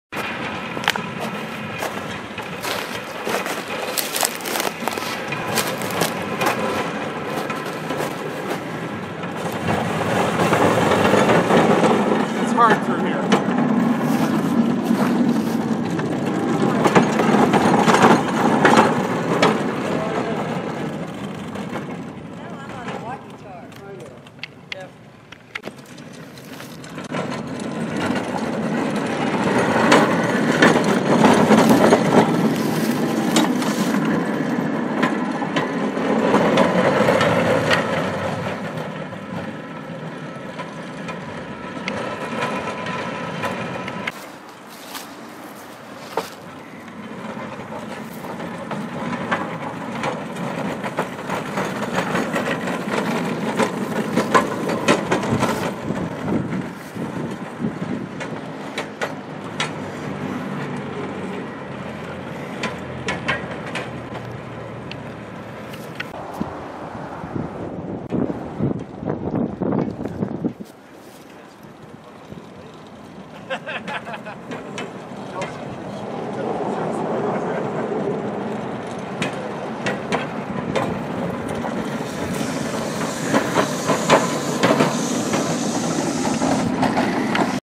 FEBT Hand Car at Robertsdale, PA 11-26-22
At the Friends of East Broad Top Museum.